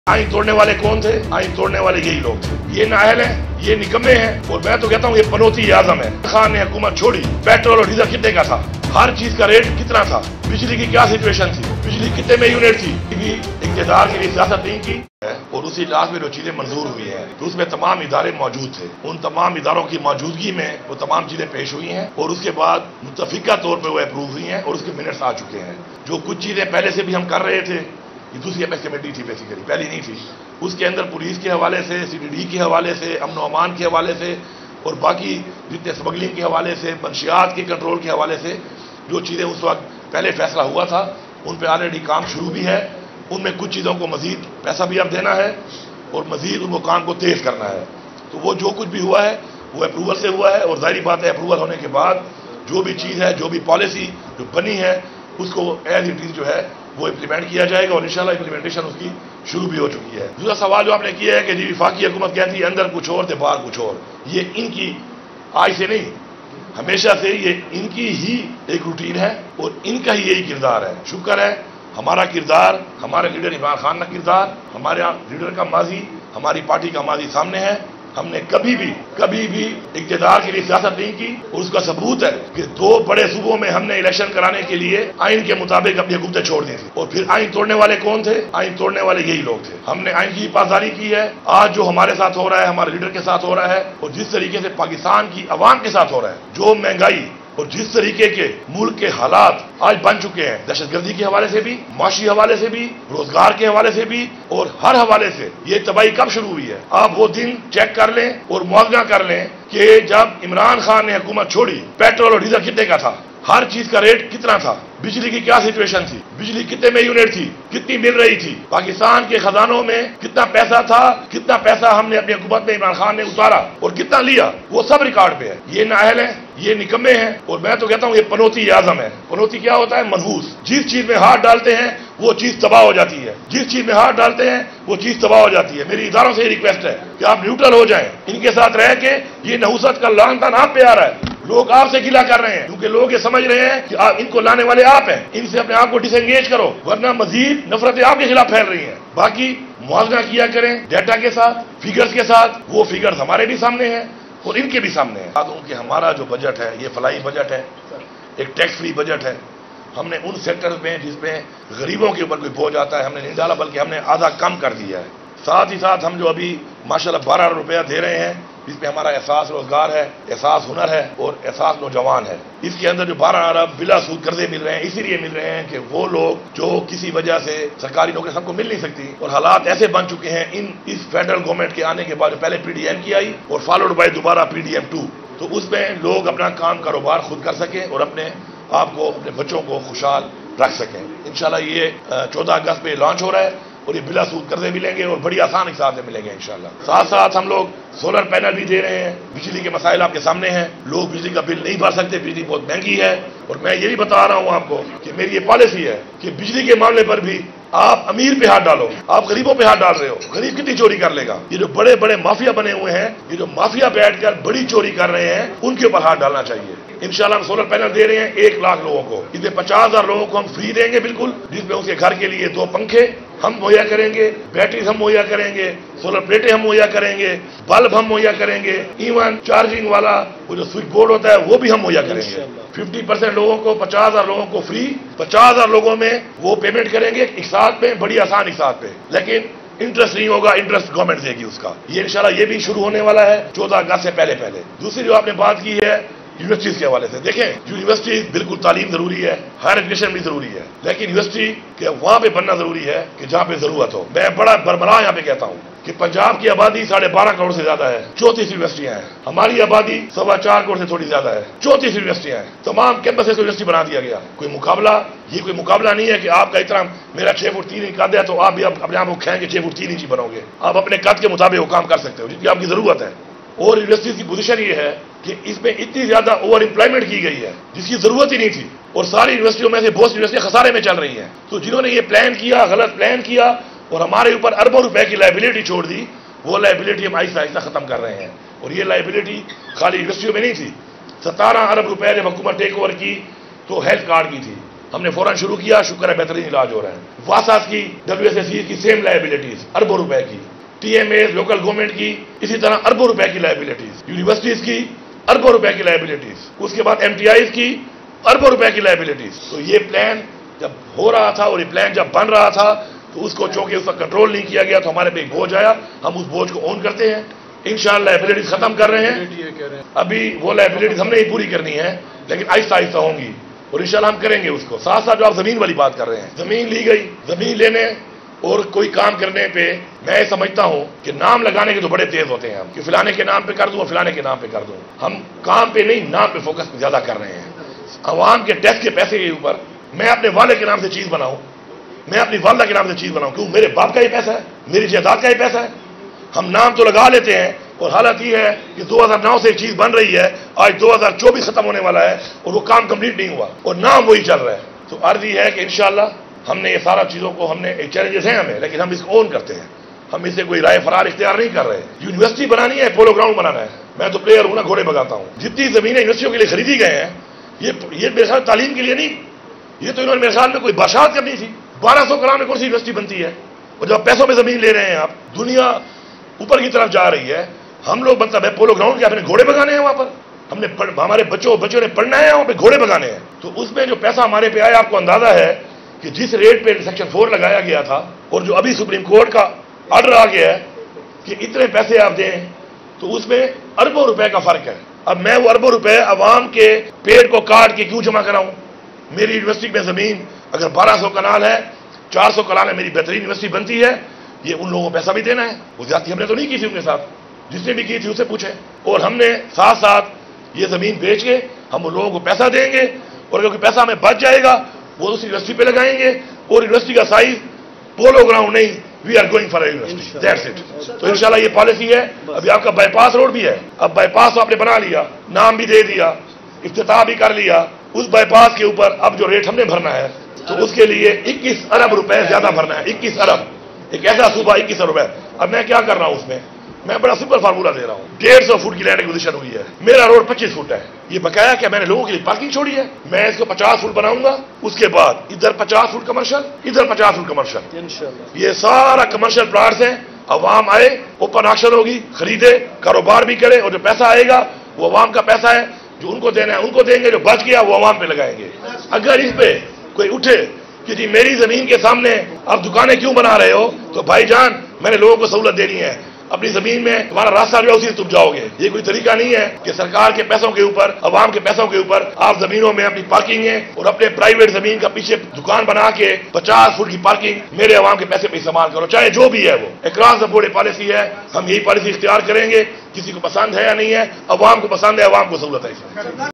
🔥 PTI Leader Ali Amin Gandapur Incendiary Press Conference! 🚨 Critique of Shehbaz Government
یہ نااہل‘منحوس‘پنوتی۔۔ادارے نیوٹرل ہو جائے ہیں ۔۔۔شہباز حکومت نے عوام کی زندگی اجیرن بنادی ہے ۔۔ رہنما پی ٹی آئی اور وزیر اعلیٰ خیبر پختونخوا علی امین گنڈاپور کی  آگ اگلتی پریس کانفرنس
🔥 PTI Leader Ali Amin Gandapur’s Incendiary Press Conference! 🚨 | "Incompetent, Scumbags | Neutral Institutions | Critique of Shehbaz Government & Its Impact on People 🇵🇰 | Imran Khan & Gen Asim Munir Discussion